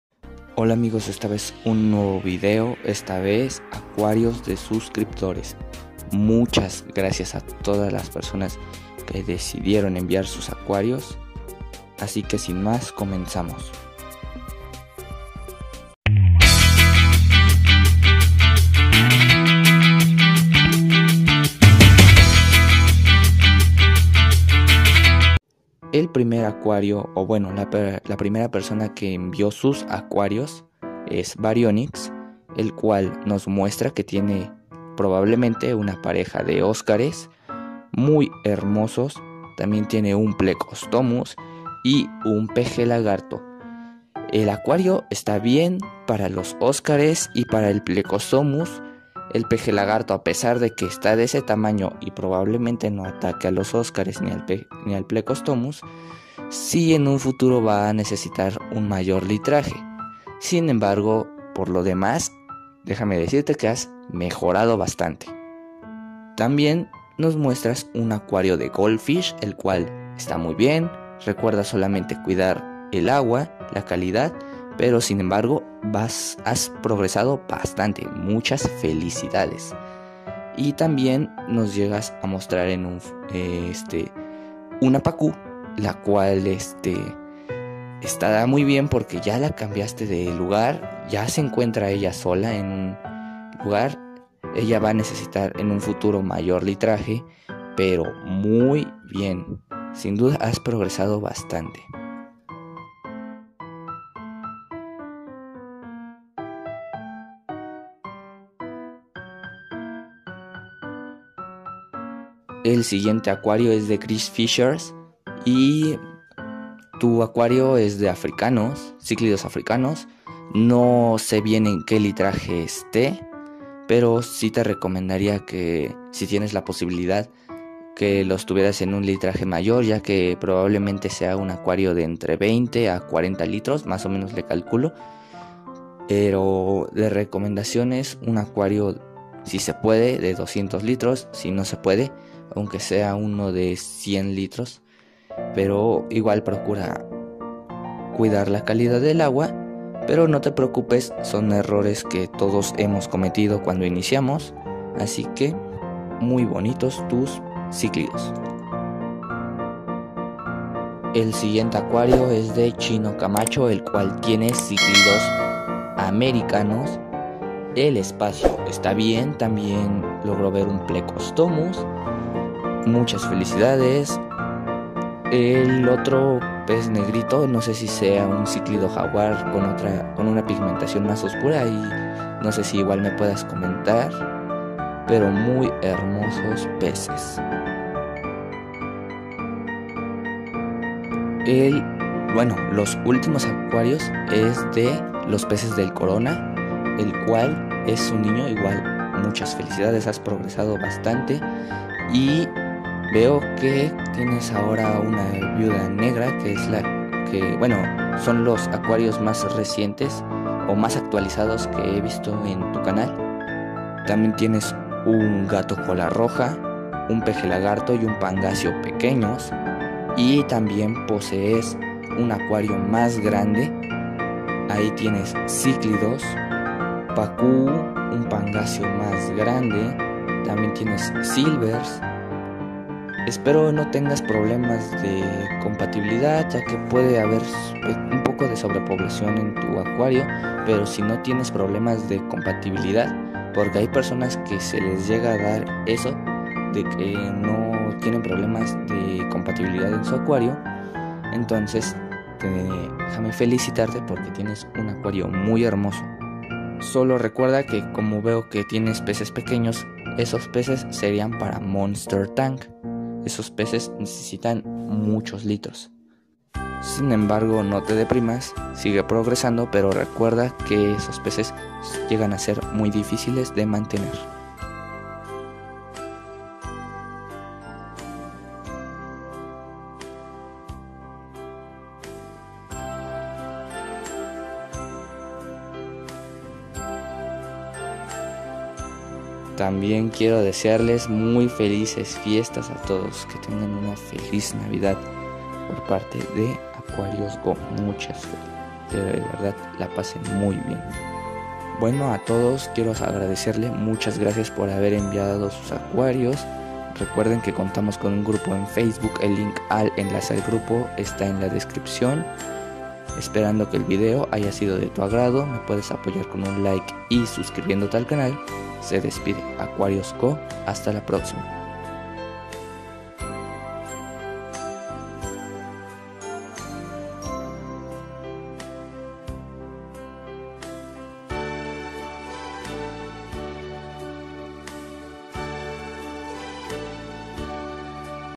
ACUARIOS DE SUSCRIPTORES #5 (Ciclidos, pejelagarto, pangasio, goldfish y más)
Gracias a todos por enviar sus acuarios y recuerden tener una feliz Navidad y felices fiestas 🎅⛄🎄